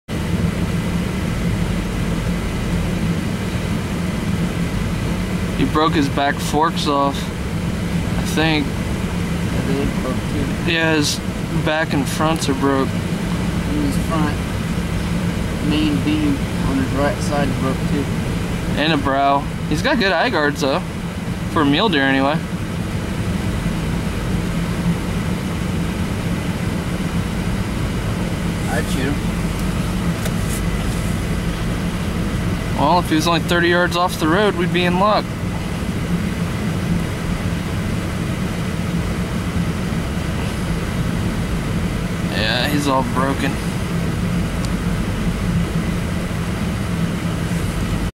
Our River Property.
We do have property other than our river ground, just everyone else thinks they can do what the F... they want on it too! It is sad that some people, I would call them Hunters but you have to be kidding me, will go to any length, even drive our Draws in vehicles, to push Deer back on to their property. They should have there hunting rights revoked! Well enough of people that do not deserve respect,at least not from me. My Son was up north and set this morning to try to fill his Archery tag, HE parked the truck and walked in 3/4 mile the way you should hunt! He ran into this Buck on us before crossing on the neighbors ground this morning after freezing there butts off this morning.